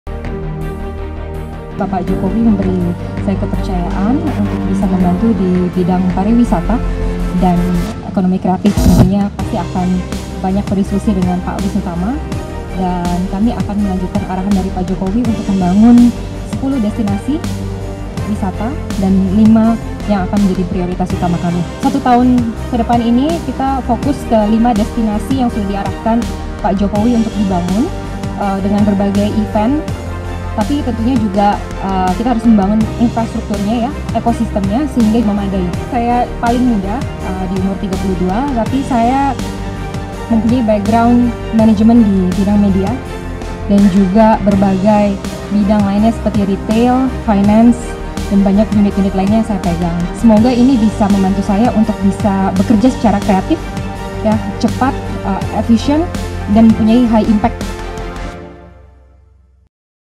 Angela Tanoesoedibjo Wakil Menteri Pilihan Jokowi yang Cantik, Muda dan Cerdas
Wasekjen Partai Perindo Angela Tanoesoedibjo diberi kepercayaan oleh Presiden Joko Widodo untuk menjadi Wakil Menteri Pariwisata & Ekonomi Kreatif - Kabinet Indonesia Maju periode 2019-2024

Dikenal sebagai sosok milenial yang penuh dengan prestasi, Angela akan berfokus untuk membangun 10 destinasi wisata dan memajukan ekonomi kreatif di Indonesia

Selamat mengemban tugas negara 🇮🇩